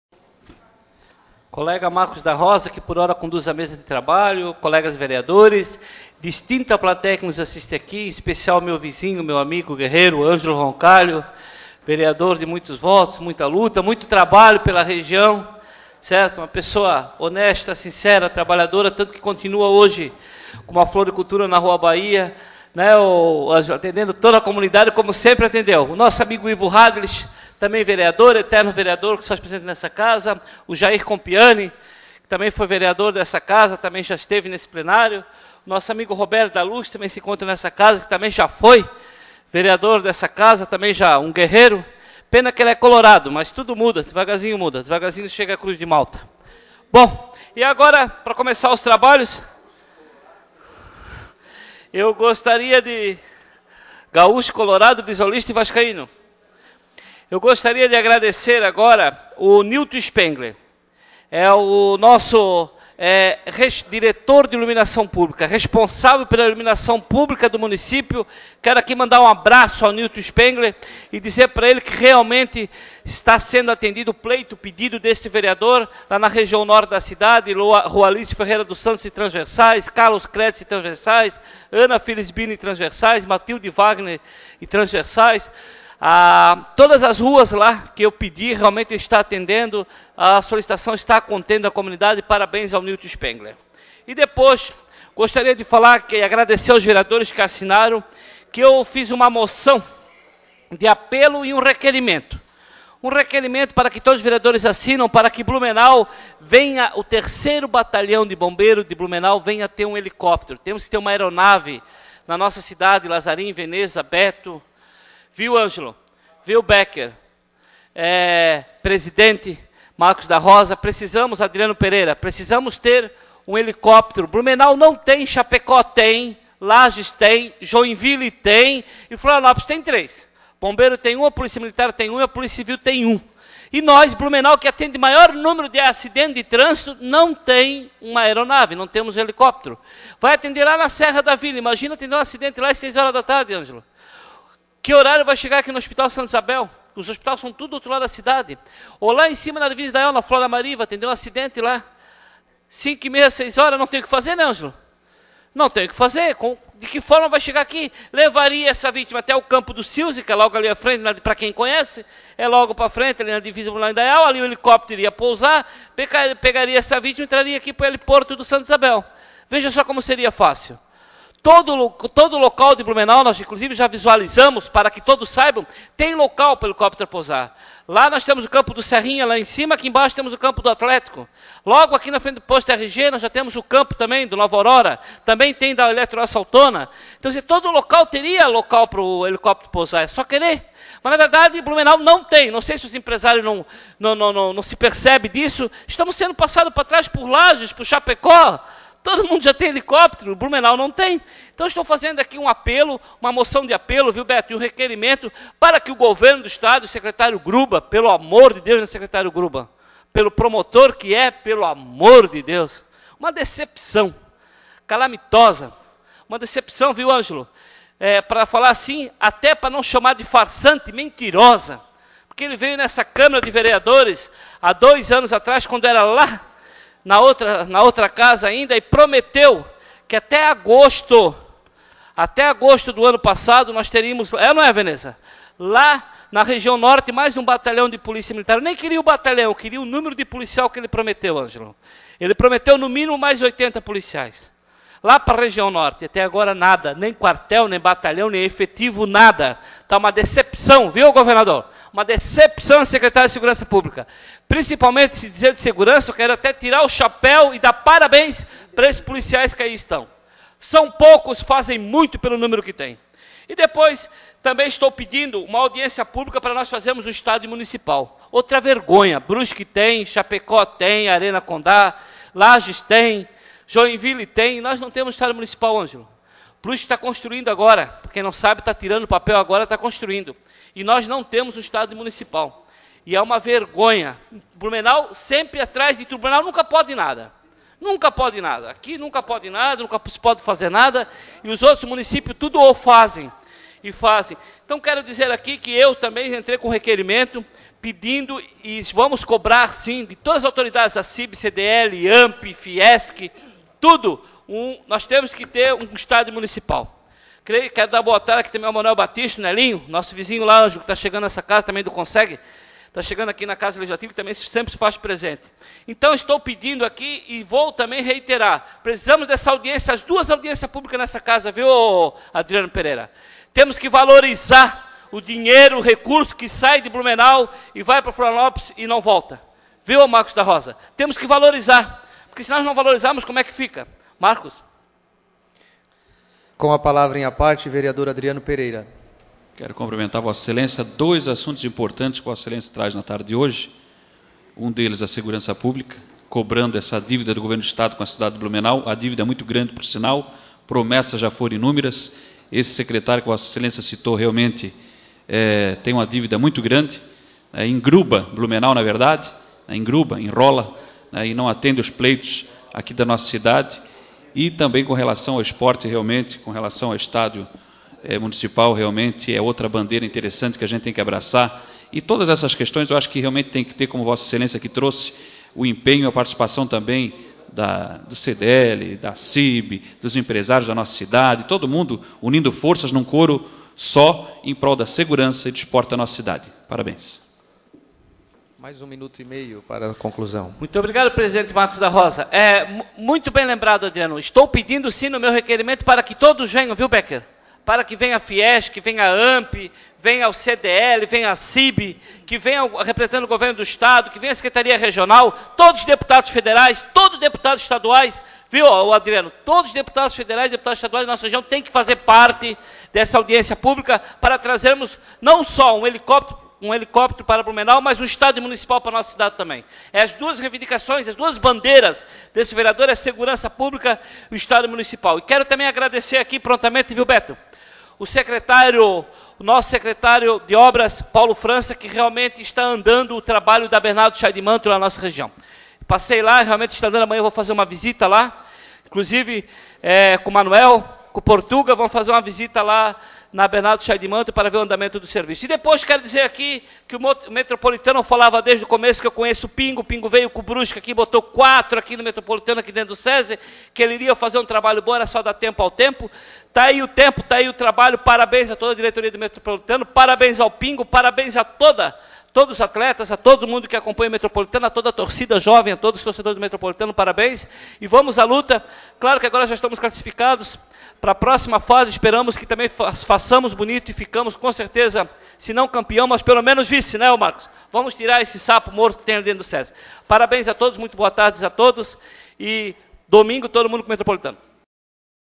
Pronunciamento do Vereador Zeca Bombeiro
O vereador Zeca Bombeiro (SD) agradeceu a todos os vereadores que assinaram sua moção de apelo, solicitando que Blumenau receba um helicóptero para que os Bombeiros possam atender ocorrências graves no município. Lembrou que as cidades de Lages, Chapecó e Florianópolis já possuem aeronaves e Blumenau ainda não. Comentou que conta com o envolvimento do secretário Estadual de Segurança Pública, César Augusto Grubba, para que demanda seja atendida. Ressaltou ainda que espera que não fique na promessa como aconteceu em anos anteriores sobre a instalação de um batalhão de Polícia Militar na região norte, que deveria ter acontecido em agosto de 2014 e até o momento Blumenau sequer recebeu reforço de efetivo. Em outro momento disse que entrou com um requerimento pedindo a realização de uma audiência pública para a construção de um estádio municipal. Ressaltou que vai solicitar a participação de entidades como Acib, Ampe, CDL, Fiesc, deputados estaduais e deputados federais para que a solicitação seja atendida. Ao final do seu discurso agradeceu ao secretário de Obras Paulo França. Disse que a obra de pavimentação da rua Bernardo Scheidemantel, no Testo Salto, está evoluindo e nos próximos dias irá acompanhar os trabalhos. Parabenizou o Clube Atlético Metropolitano pelos excelentes resultados conquistados durante o Campeonato Catarinense e pediu a participação dos blumenauenses para apoiar o time neste momento decisivo.